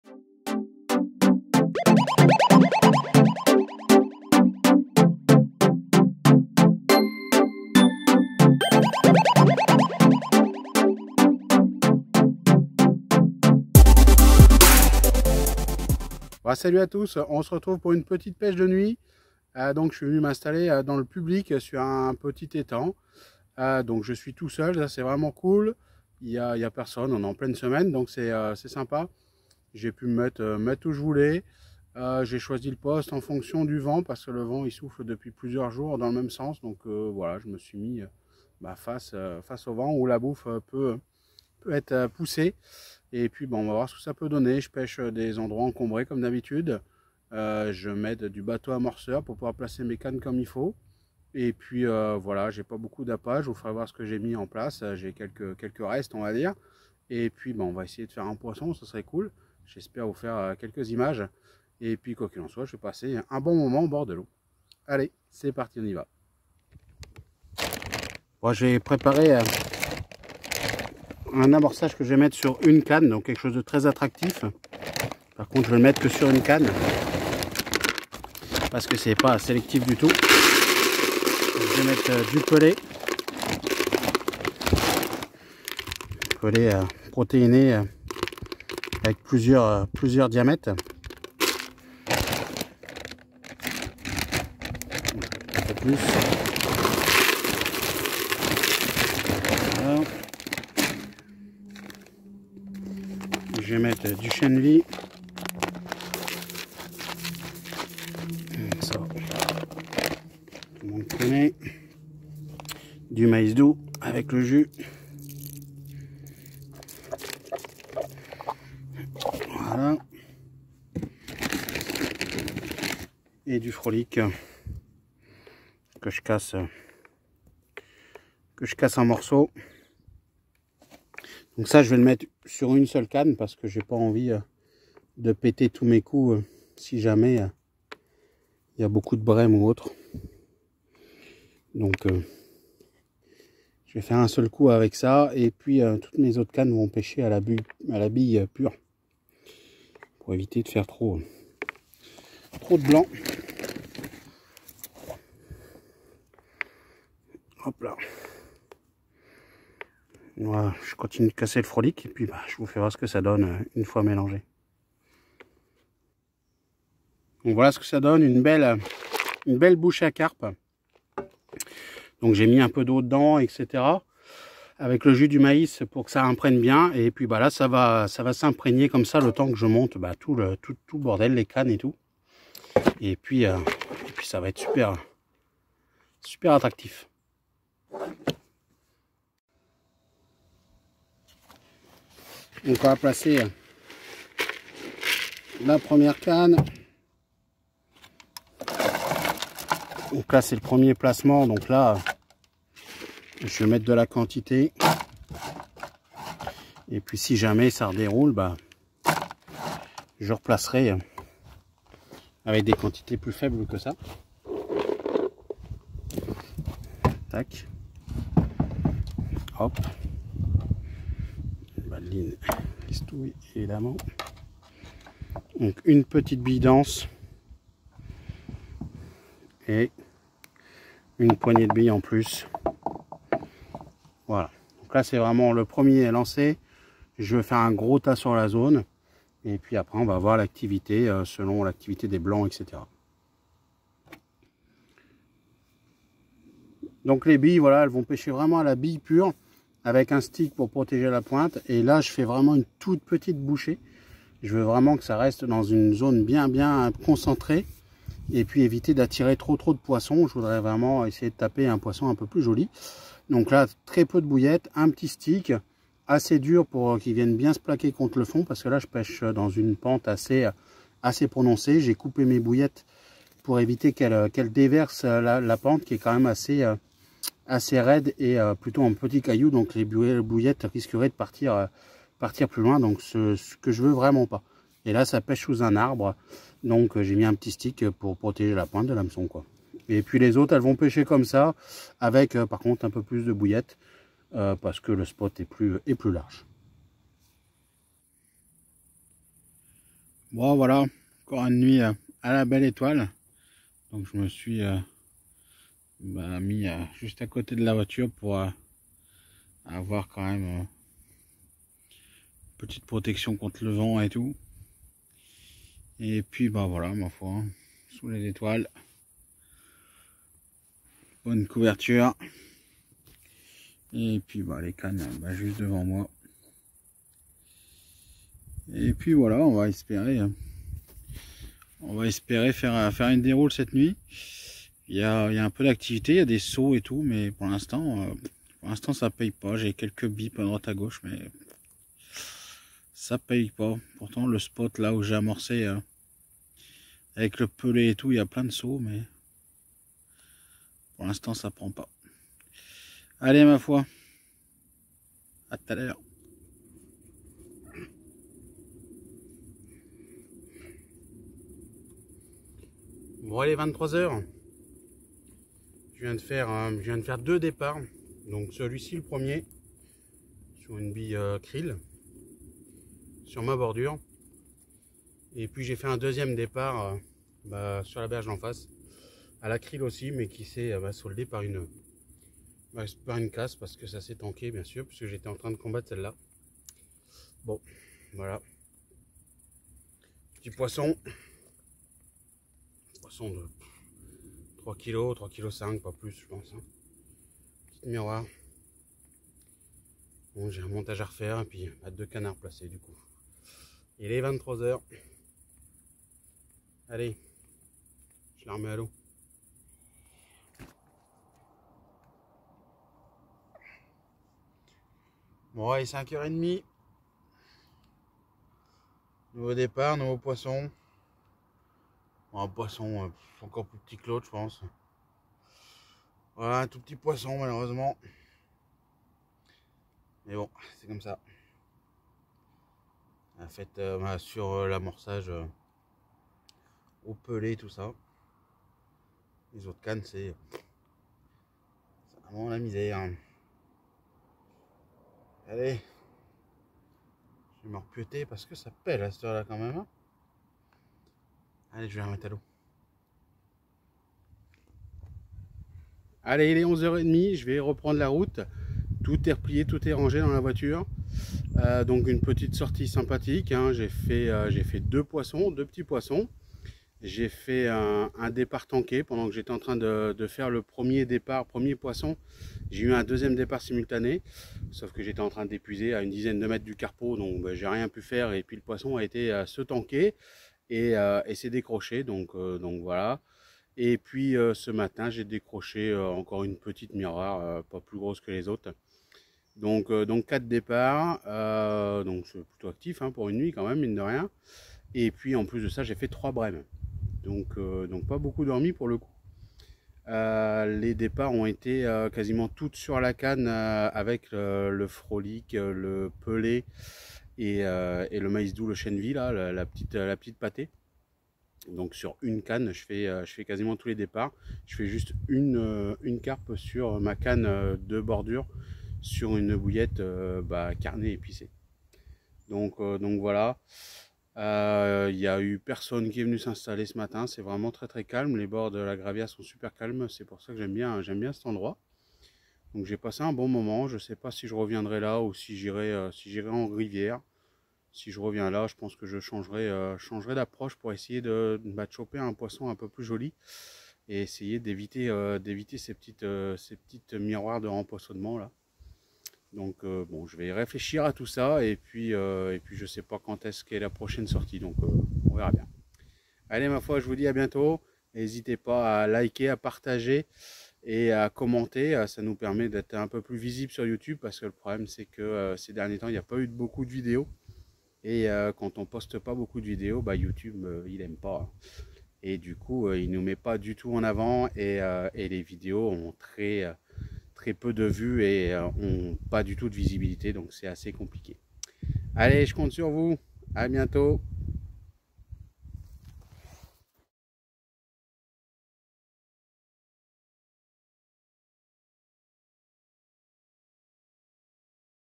Une nuit à la belle étoile ☼ ** pêche de la carpe **
Retrouvez Seb dans une petite gravière.